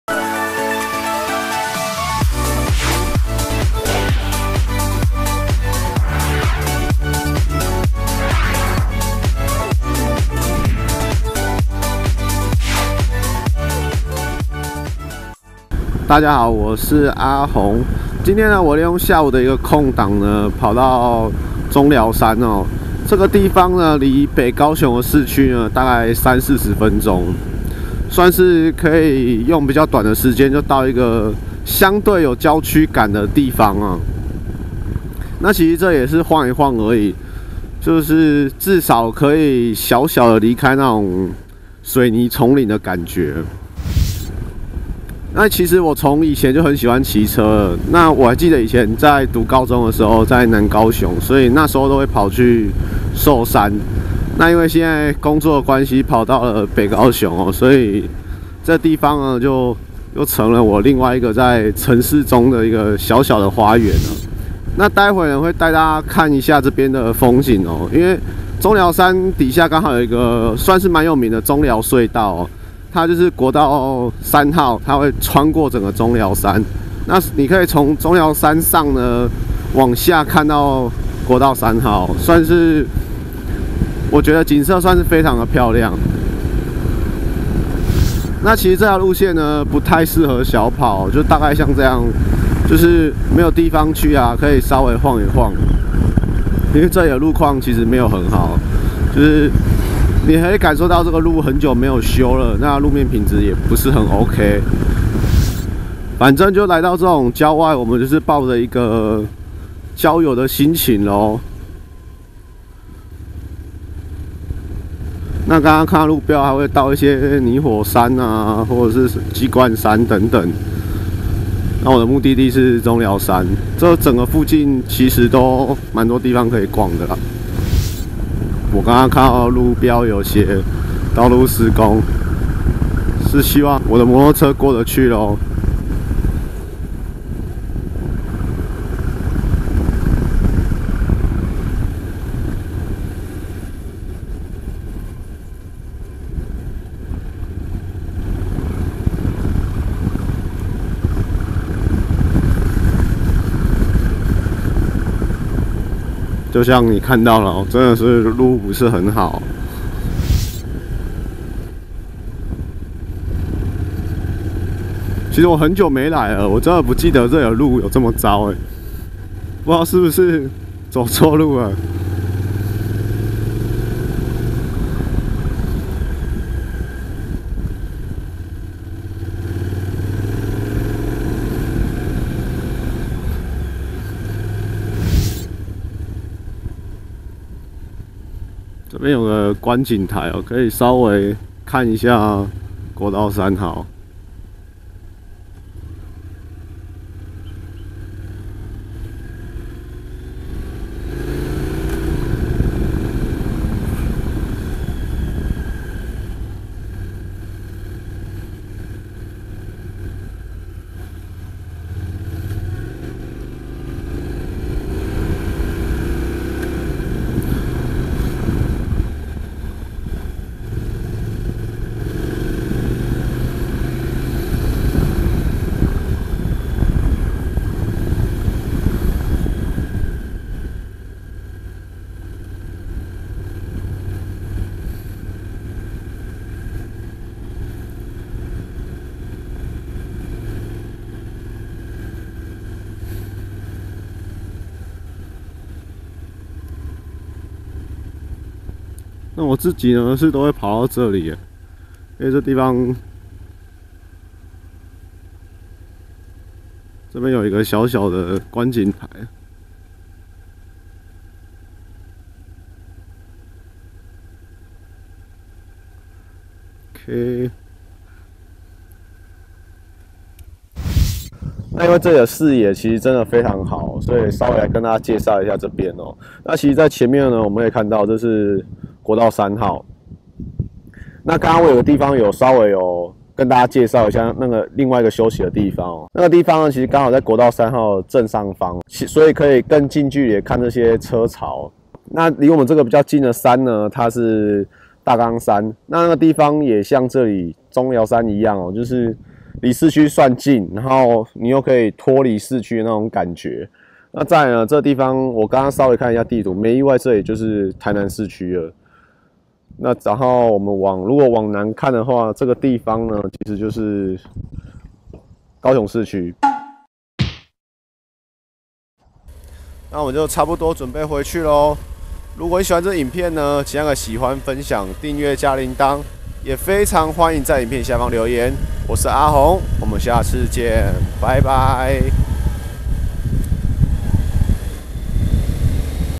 【Vlog】-7 中寮山晃晃 北高雄的後花園 RV的重機路線
下午剛好有個幾個小時的空檔，想說很久沒去中寮山了，就跑去晃晃囉！